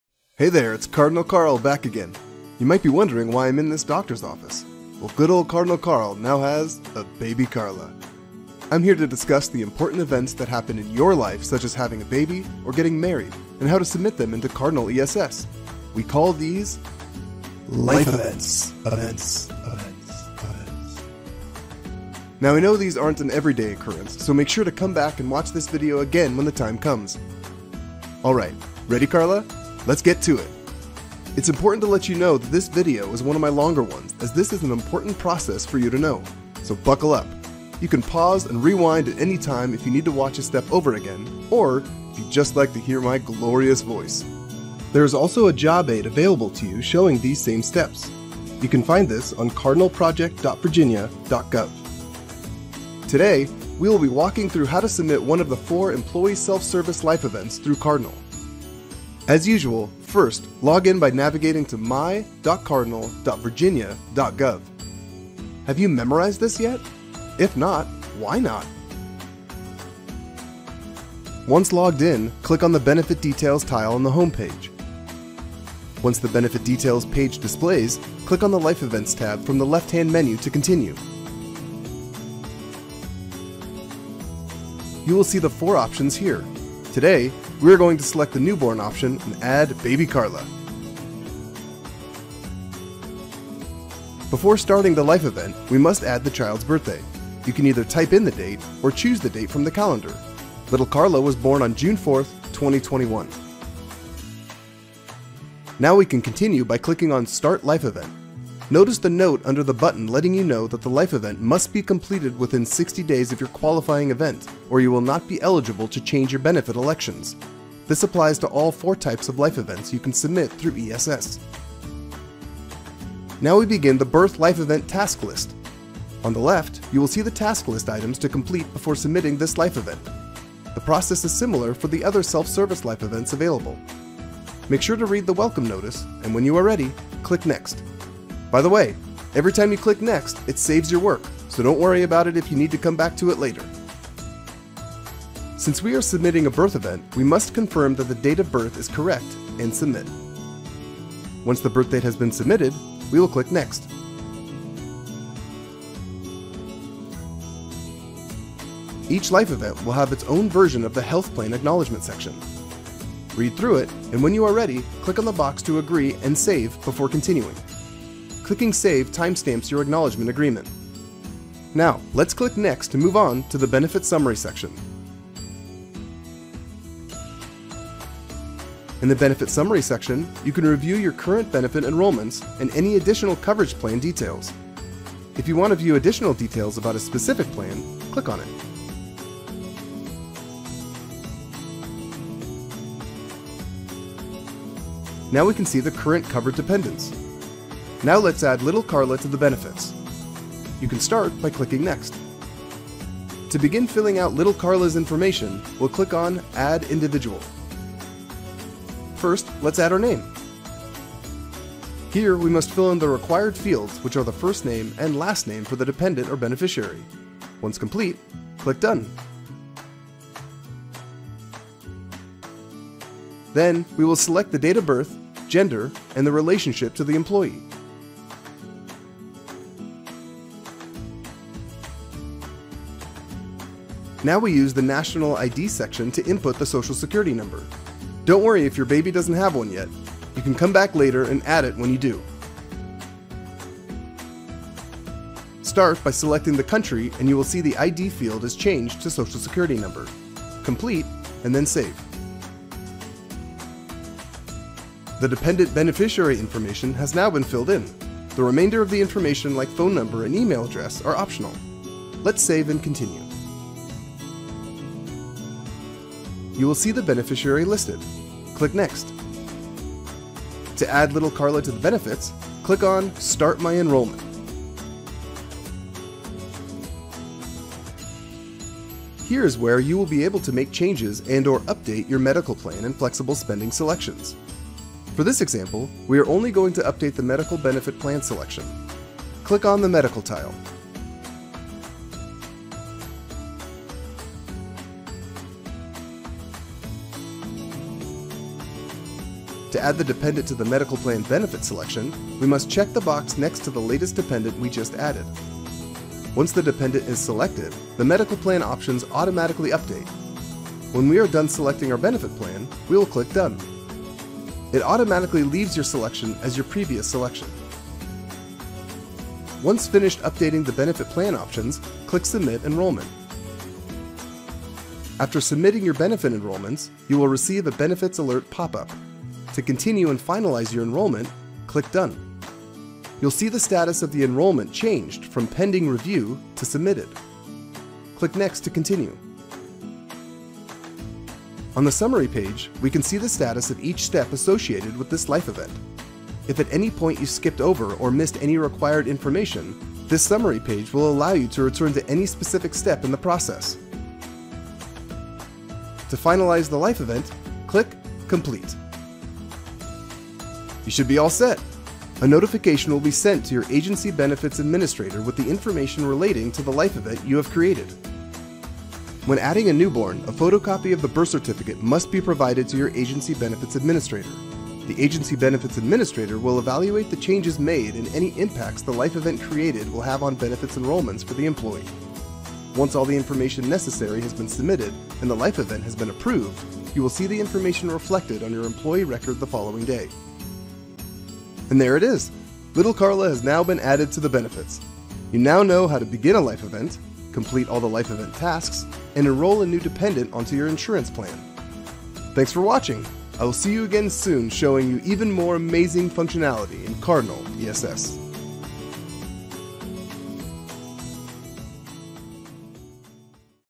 How to Create a Life Event
This tutorial video demonstrates the process used to create a Life Event using the Cardinal Human Capital Management (HCM) application's Employee Self-Service functionality.  Life Events are those mid-year qualifying events that allow you the opportunity to make c.hanges to your benefits enrollments such as marriage or birth of a child.